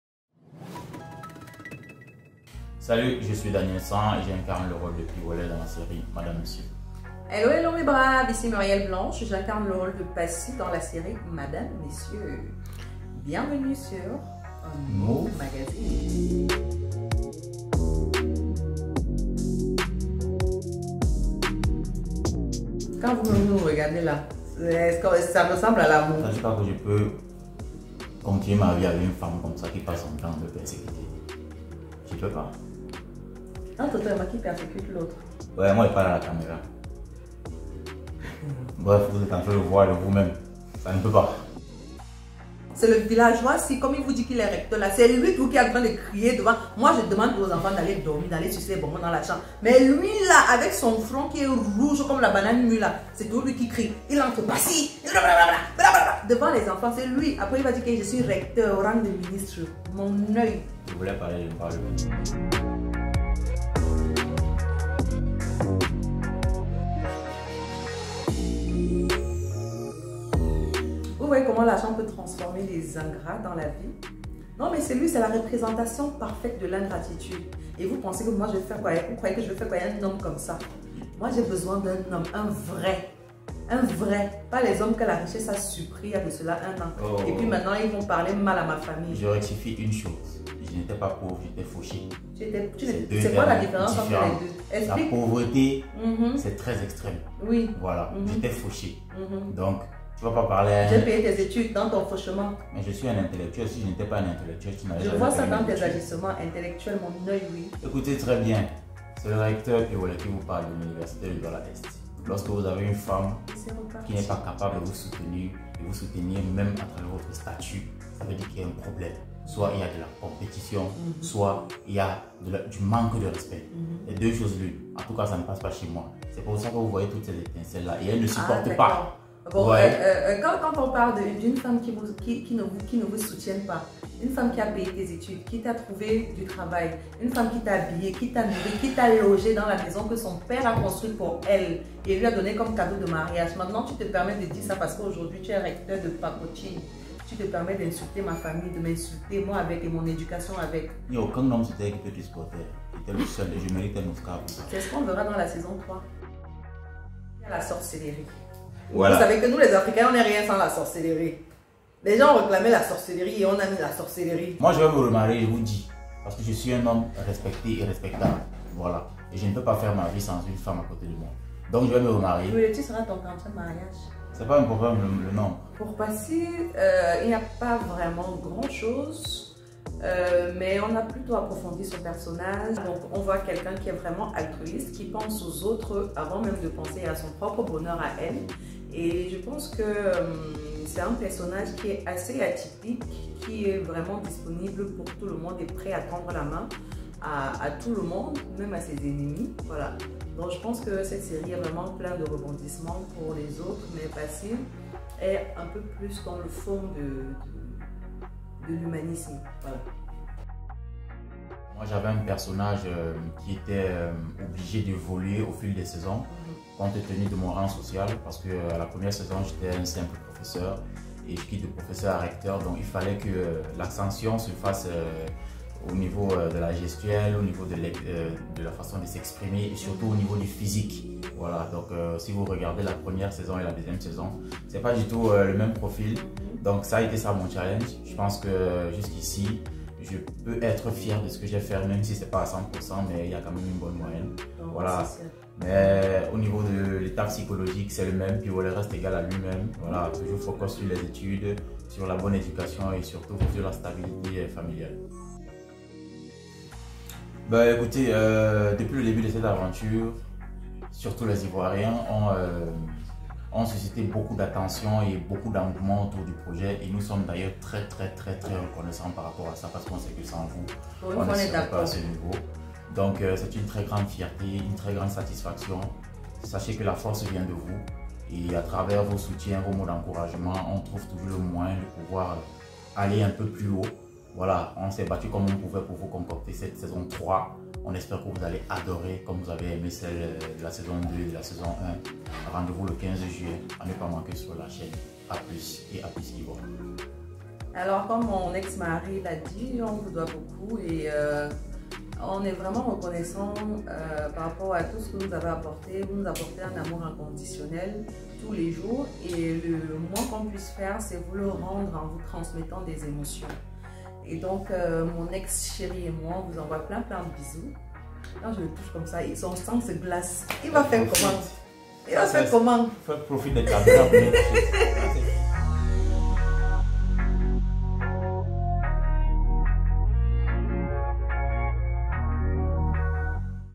MURIEL BLANCHE ET DANIEL NSANG NE SONT PAS PRET POUR LA RECONCILIATION / MADAME MONSIEUR S3
Bonjour
Eux c'est piwole et passy ou @daniel_nsang et @muriel_blanche, en tout cas appelé les comme vous le voulez...
Nous les avons surnommé thedramacouple❤️ et on les aime.
Suivez l'integralité de leur interview sur: